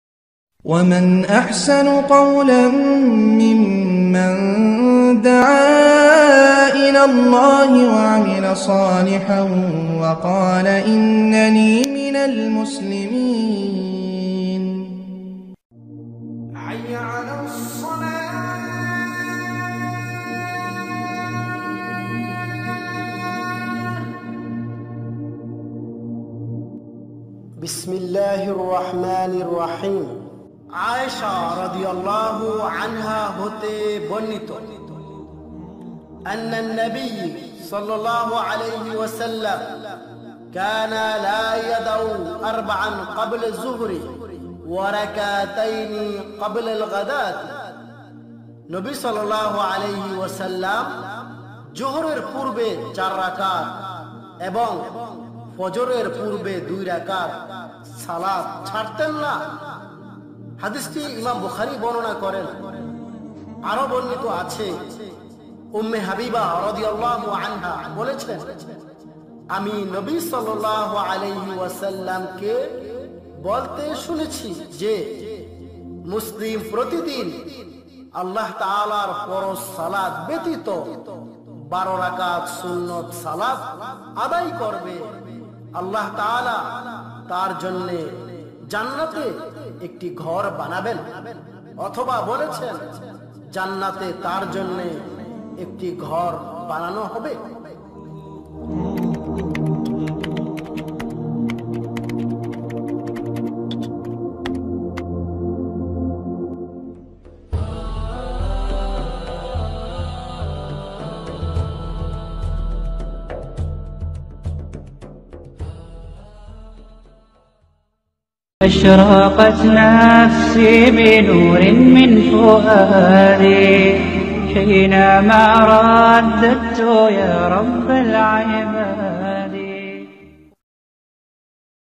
সুন্নাত সালাতের গুরুত্ব ও মর্যদাᴴᴰ┇by জামাল গাজী ইবনে সফিউল্লাহ
The importance and dignity of the Sunnah

🎙Jamal Gazi Ibn Shafiullah

👋 yeamul islam 👋